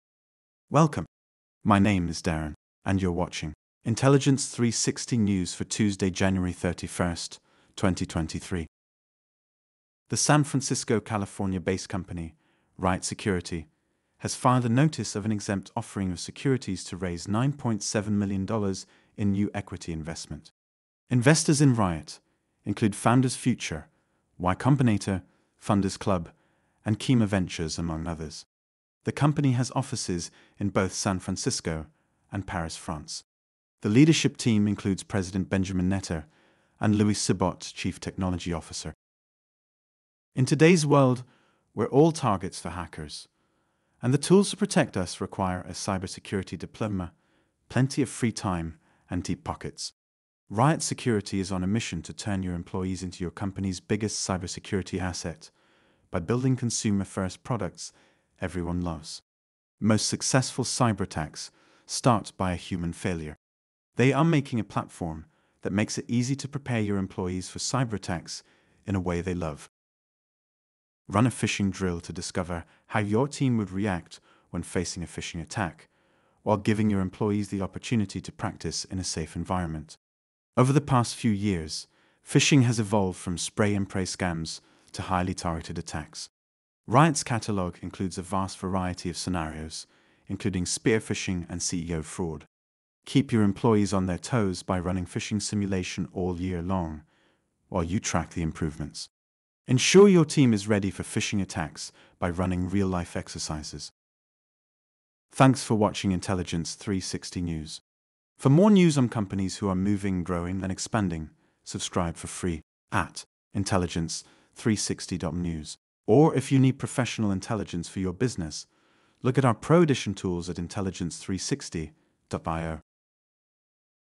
Riot Security, has filed to raise $9.7 Million Dollars in new equity investment.
According to filings with the U.S. Securities and Exchange Commission, Riot Security is raising $9,700,211.00 in new funding. Sources indicate as part of senior management President, Benjamin Netter played a key role in securing the recent investment and it will aid in aggressively expanding the company, as well as broaden and accelerate product development. In todays world, most successful cyberattacks start by a human failure. Were making the platform that makes it easy to prepare your employees for cyberattacks, in a way they love. Source: intelligence360.io and intelligence360.news